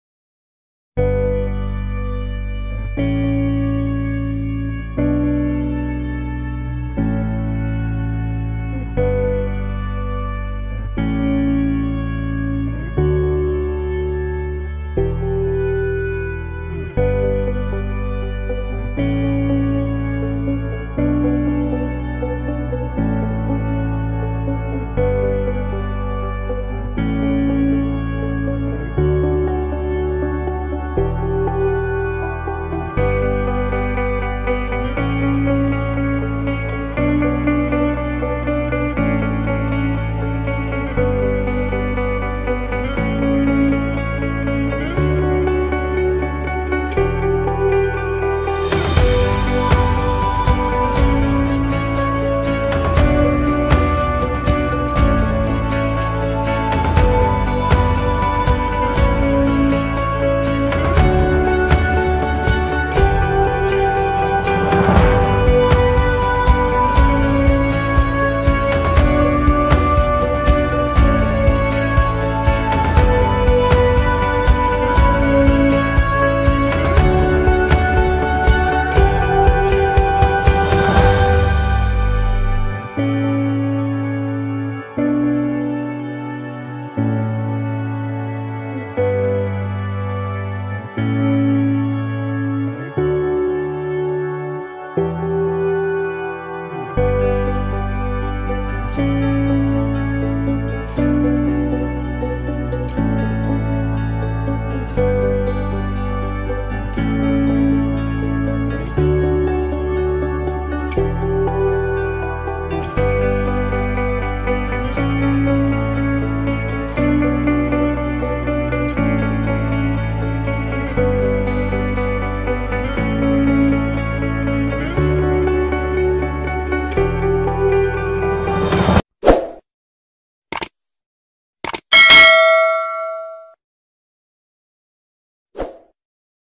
how to make a room heater from an old electric mosquito sprayer low voltage, must watch
get an old mosquito sprayer and do this👆